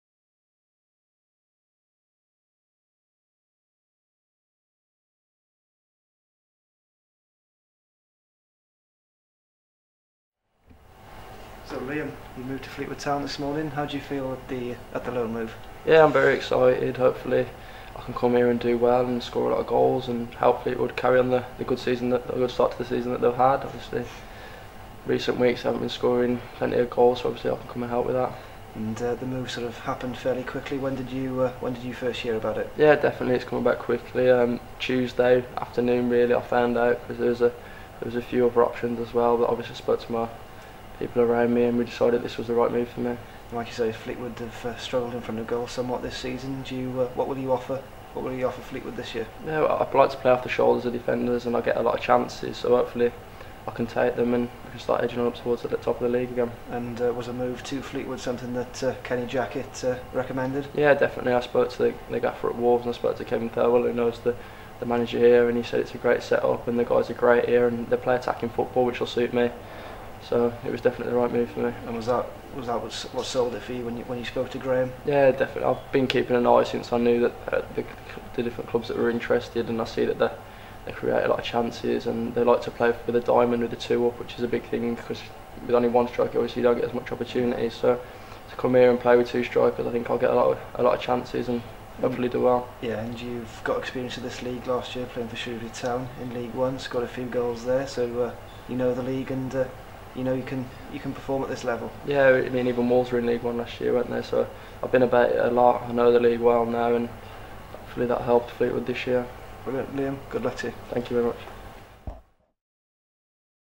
WOLVES STRIKER: McAlinden aims high with Fleetwood
Our new loan striker Liam McAlinden speaks to the Club's media team after penning a three month loan deal from Wolverhampton Wanderers